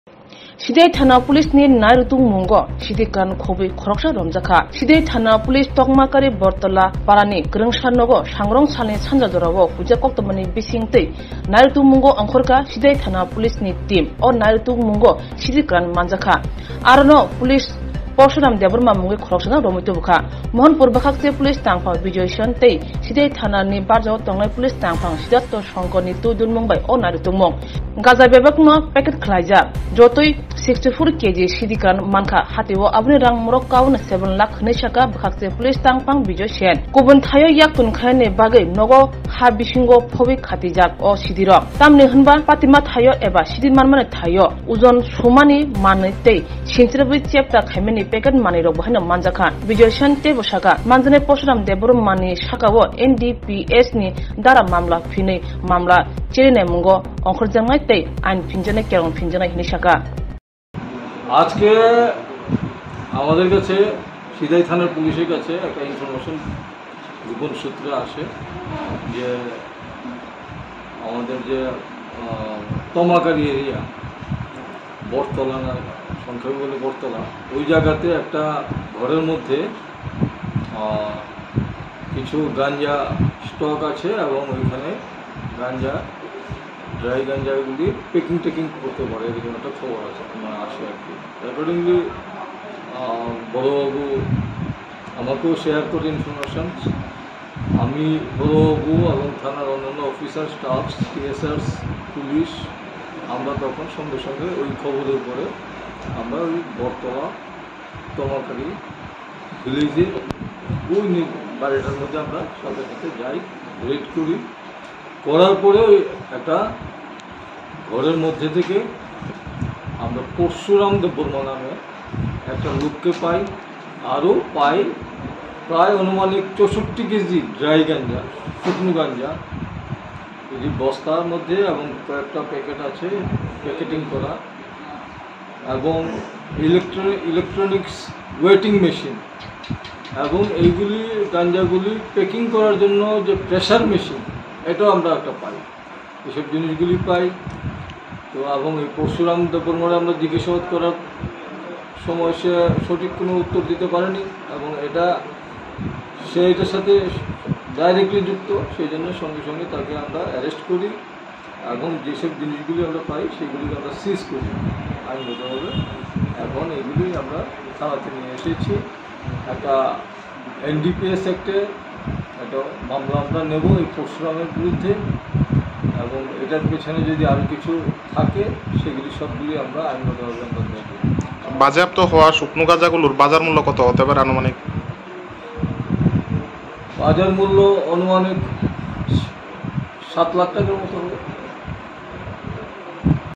Siddi Kwran Khobwi Khoroksano Romthani Sucessful Sidai PSni Team
The police team of Sidhai police station raided a house in Battala neighborhood of Tamkari on Thursday evening on the basis of information from secret sources and seized dry ganja and arrested one man.